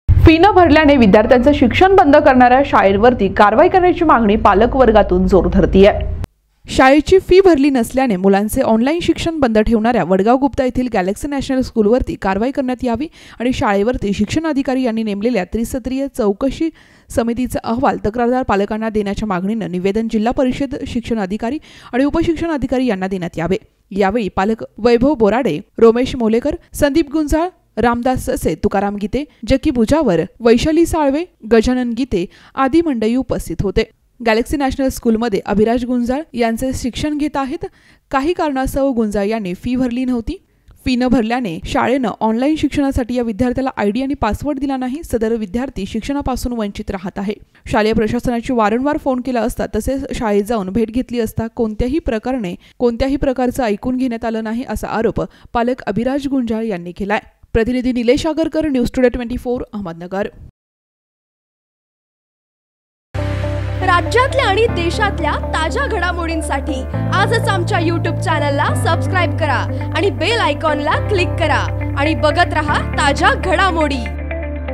अहमदनगर : फी न भरल्याने विद्यार्थ्यांचे ऑनलाईन शिक्षण बंद करणार्‍या त्या शाळेवर कारवाई व्हावी
पालकांनी दिले शिक्षणाधिकारींना निवेदन शाळेवर नेमलेल्या त्रिस्तरीय चौकशी समितीची अहवाल पालकांना देण्याची मागणी